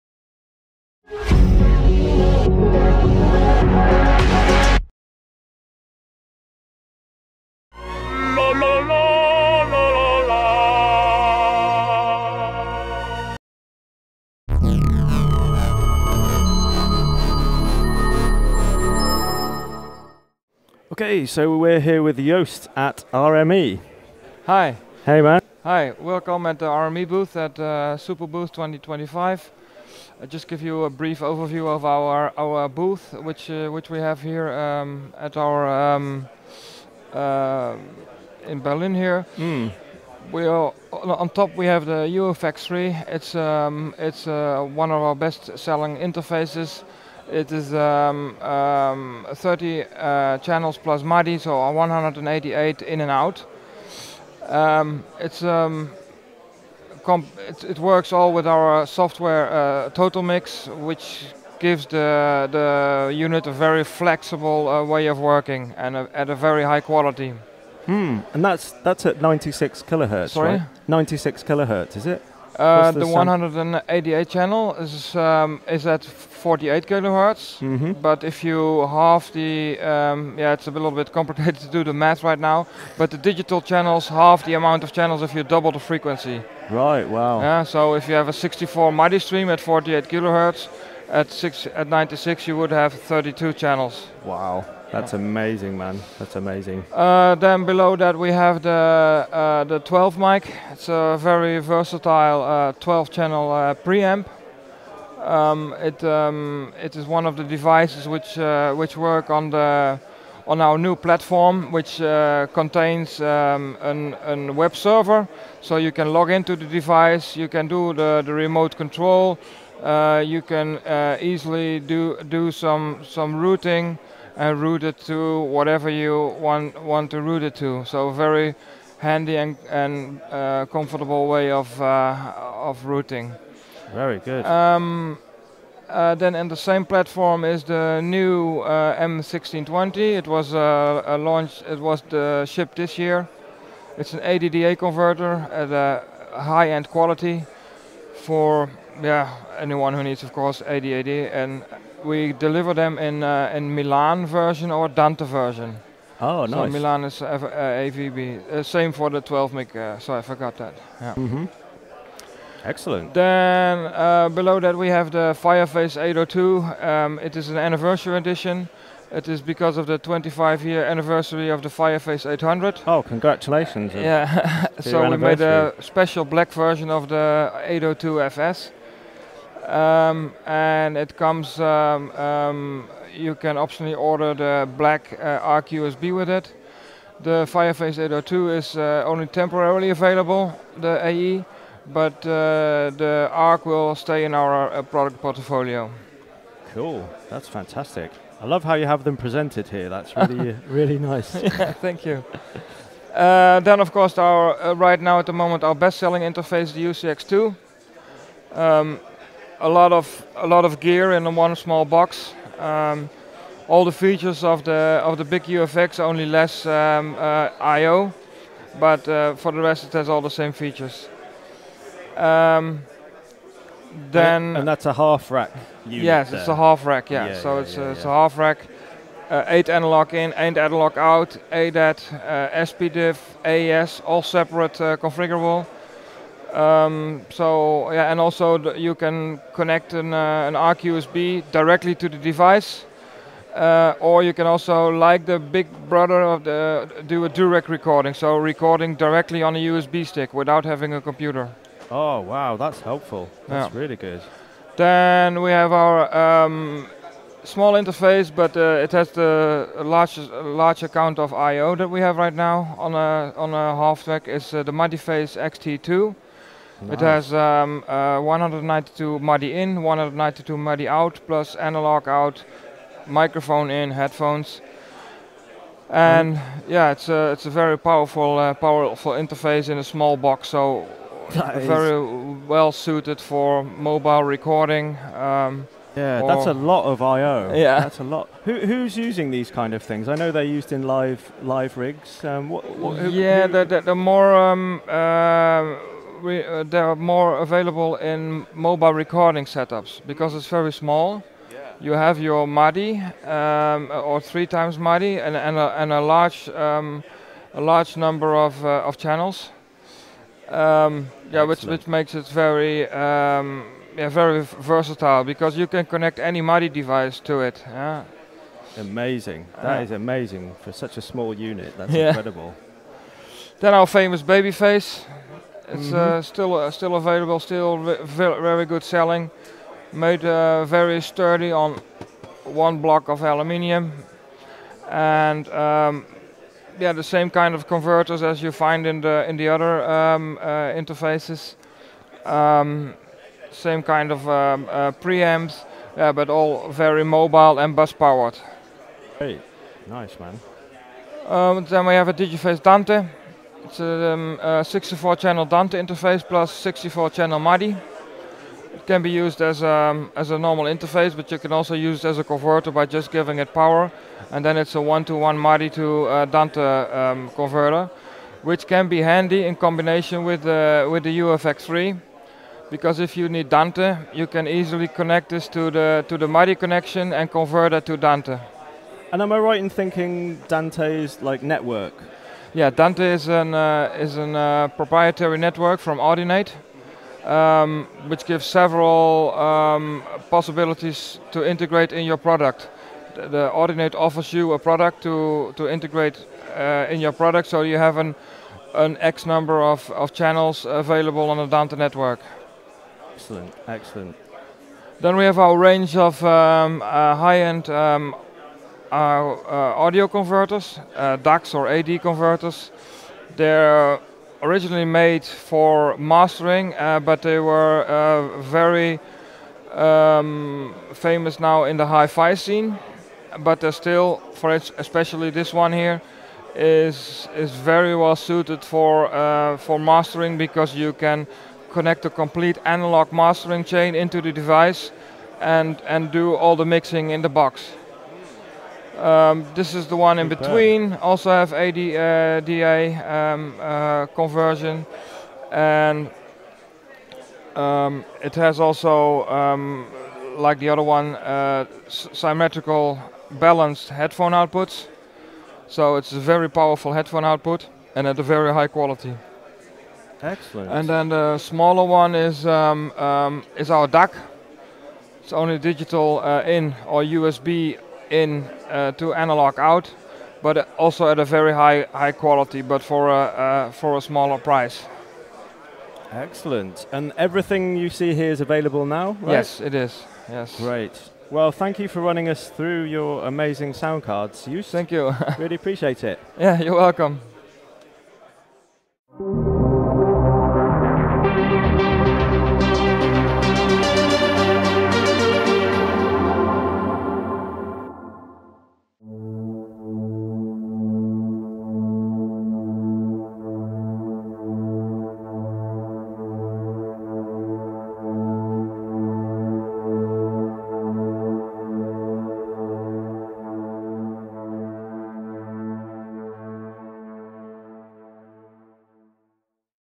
RME: Interfaces and Converters - Superbooth 25
We had a chat with *Joost* from *RME*, who showcased their latest offerings at Superbooth 2025. The *UFX3* is highlighted as a favorite, boasting 30 channels plus MIDI, and integrates seamlessly with RME's TotalMix software. Although at 48kHz it supports 188 channels, these halve when the frequency is doubled. The *12Mic* is a versatile preamp with a built-in web server, facilitating straightforward remote access and routing. Another mention was the *M1620*, a high-end ADDA converter suitable for Milan or Dante versions.

Celebrating the 25th anniversary of the Fireface 800, RME introduced the limited edition *Fireface 802* in a sleek black design with optional ARC USB. The compact yet feature-rich *UCX2* and the *Muddyface XT2* with considerable I/O options were also on display, perfect for mobile recording. Lastly, the *Digiface Dante* offers dual capabilities as an interface and a MADI to Dante converter.